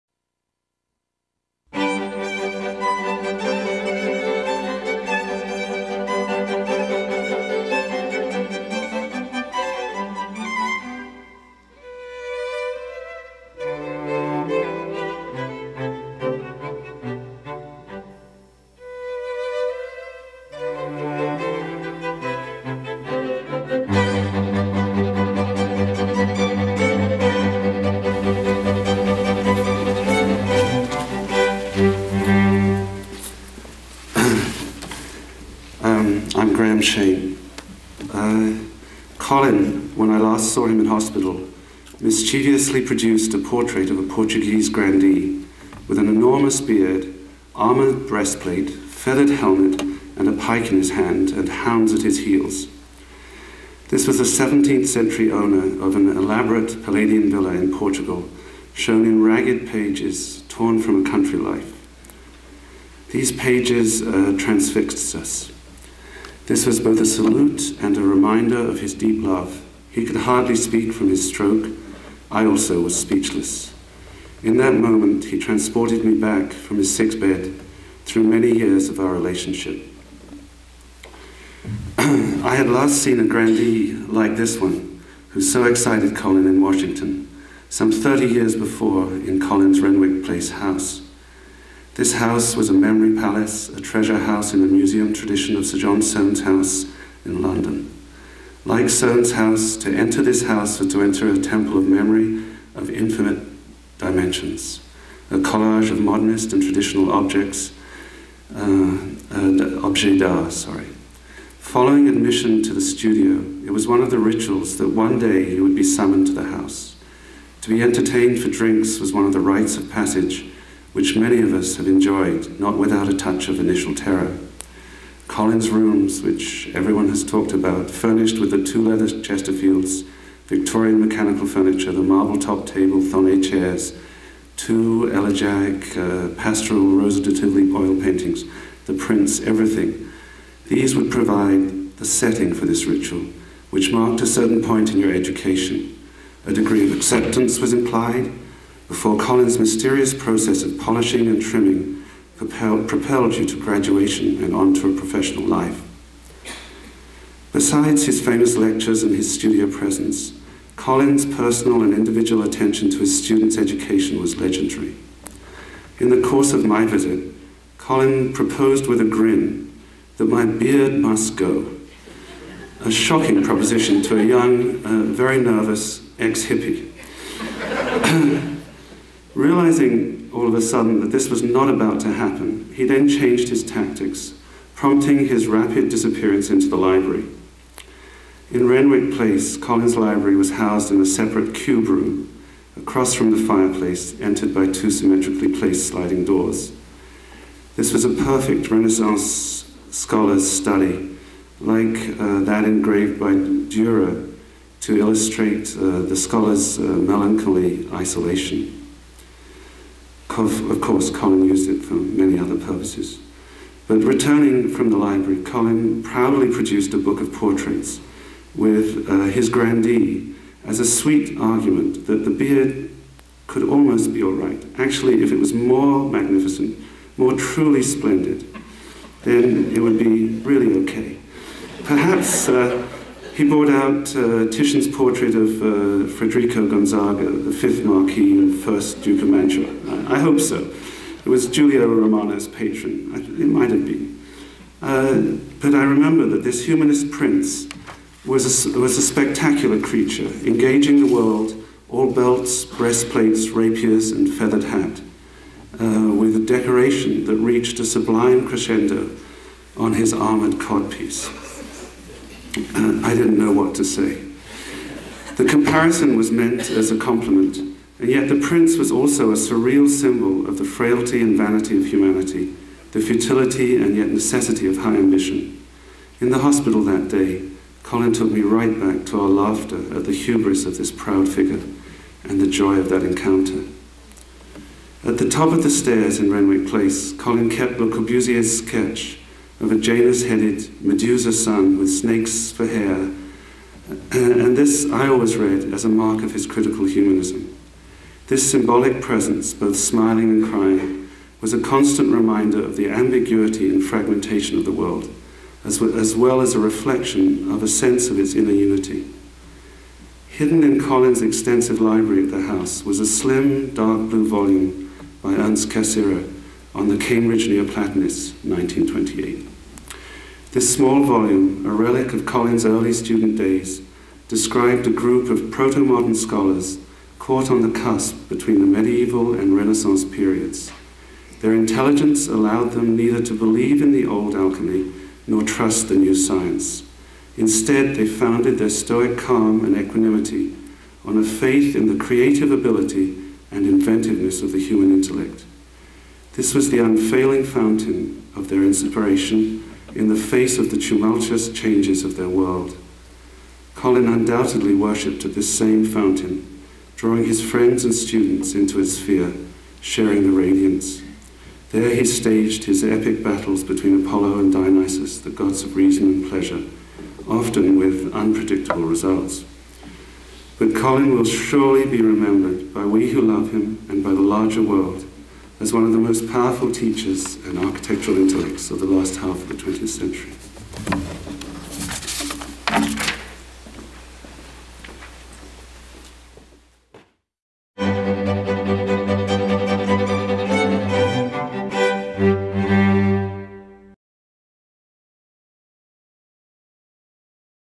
Colin Rowe Memorial, Grahame Shane speaker
A Memorial Event was held for Colin Rowe on February 6, 2000 at the Carnegie Institution of Washington, located in Washington DC.